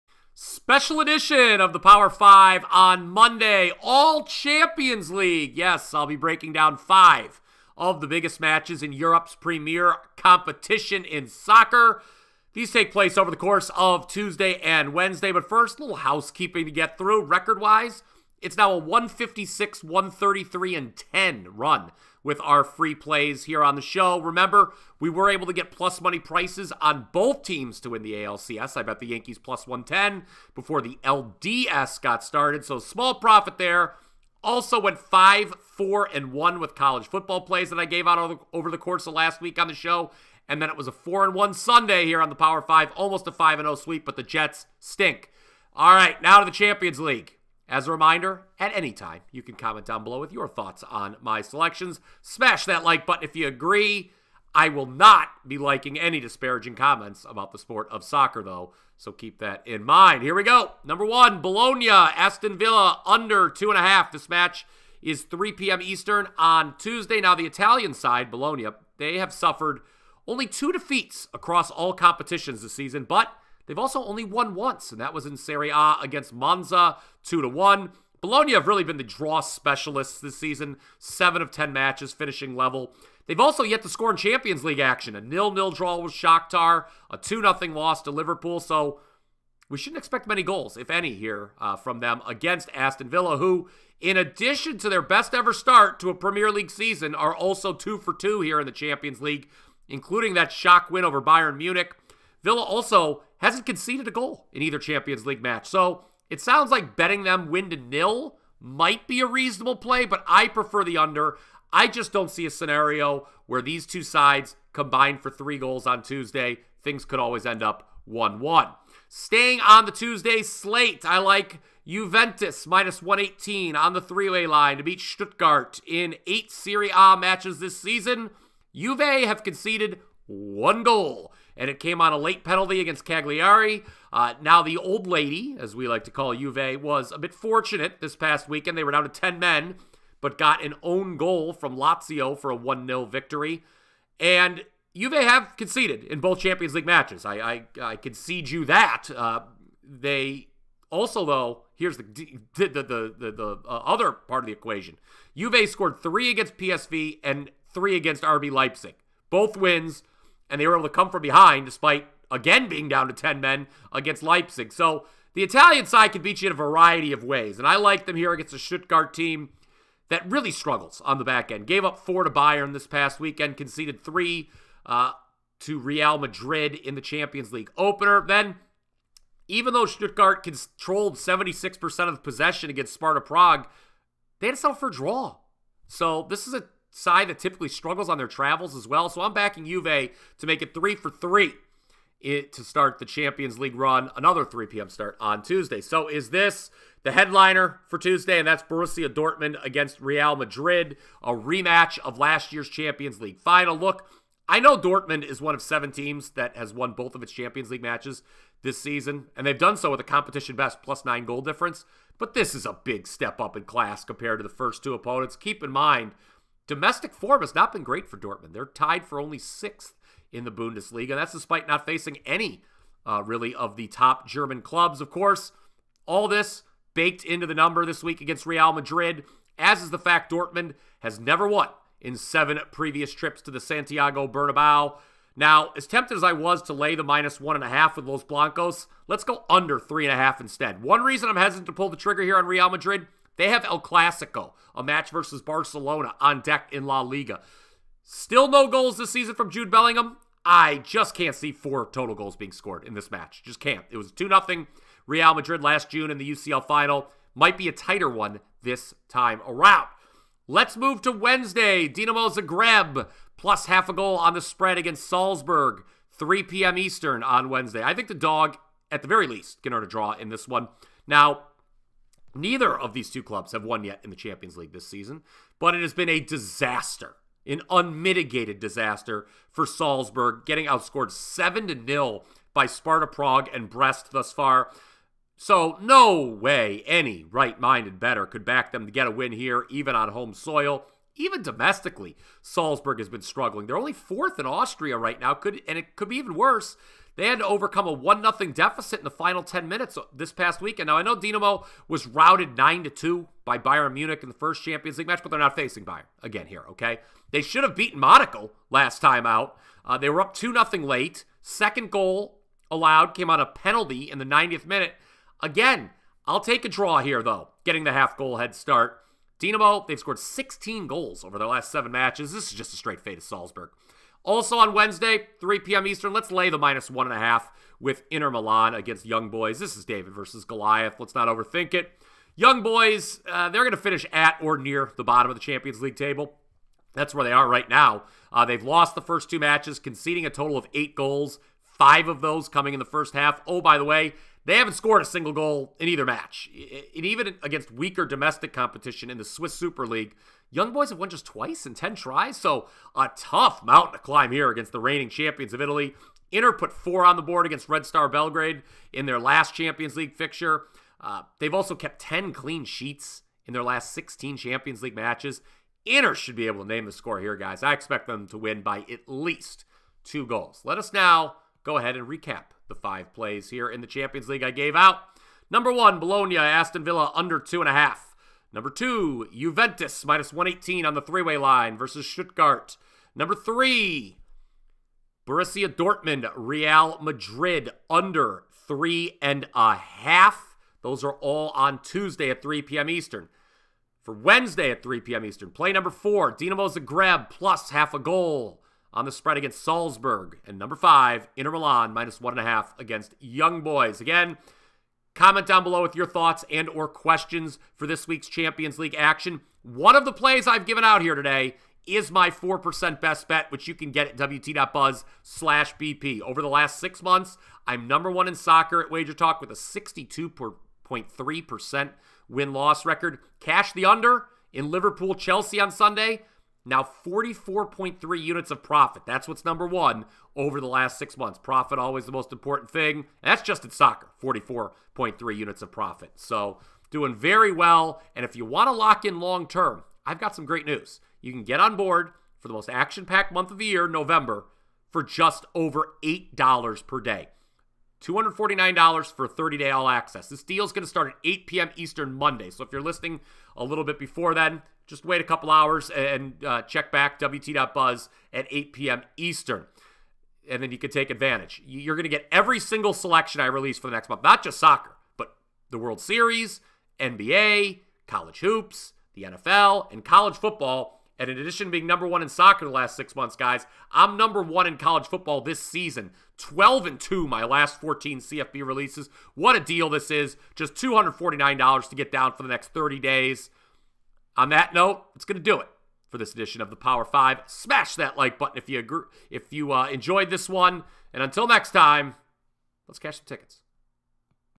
UEFA Champions League Picks and Predictions Today | Champions League Bets | Power 5 for 10/22/24
🤑 Join Bryan Power for the 10/22/24 edition of the Power 5 as he gives UEFA Champions League picks and predictions including Stuttgart vs Juventus, Bologna vs Aston Villa and Young Boys vs Inter Milan! Don't miss out!

***POWER 5 ON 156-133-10 RUN

Introduction 00:00
UEFA Champions League (Tuesday): Bologna vs Aston Villa 01:00
UEFA Champions League (Tuesday): Stuttgart vs Juventus 01:59
UEFA Champions League (Tuesday): Real Madrid vs Dortmund 03:15
UEFA Champions League (Wednesday): RB Salzburg vs Dinamo Zagreb 04:20
UEFA Champions League: Young Boys vs Inter Milan 05:22
Power 5 Monday Recap 06:10
Bryan / WagerTalk Monday Offers!! 06:50

👍 You can get free picks and betting tips direct from Vegas by following the WagerTalk handicappers on Twitter: